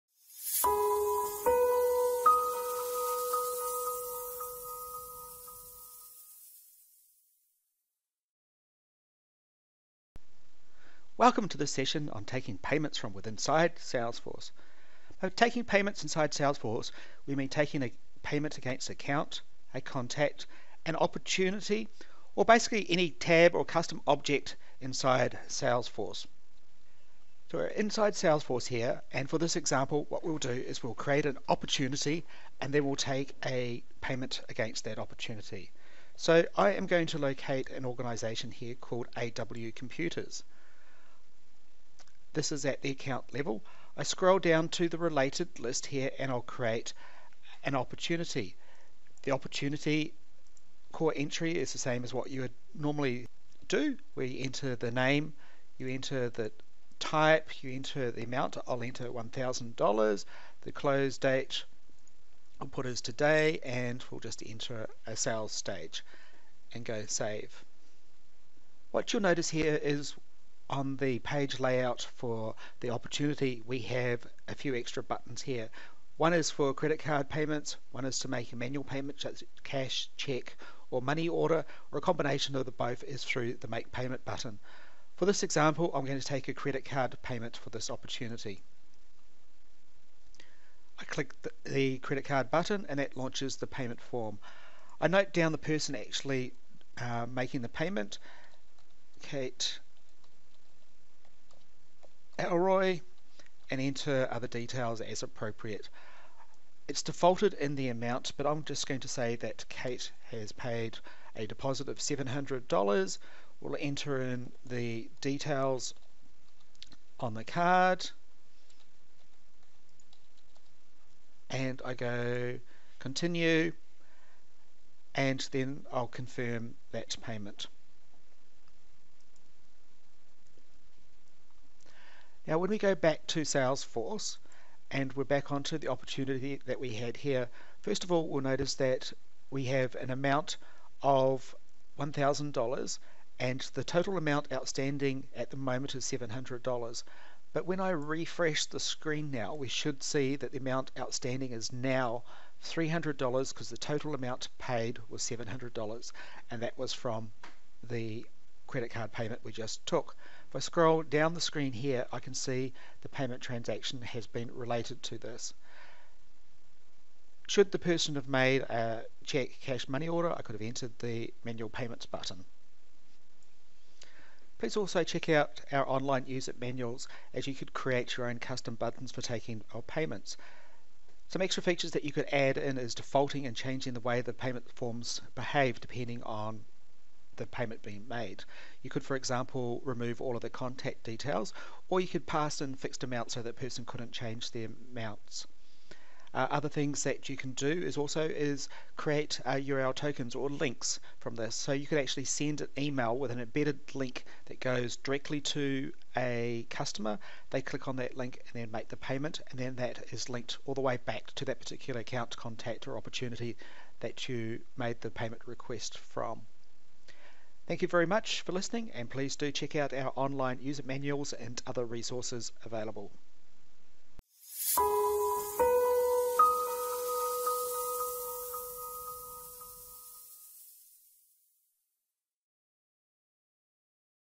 Payments Within Salesforce
Do you need to take a Credit Card or any type of payment against an Opportuntiy, Account, Contact or in fact any object in Salesforce?  This session walks you through an example of how to take a payment against an Opportunity and shares resources available for you to create your own payment buttons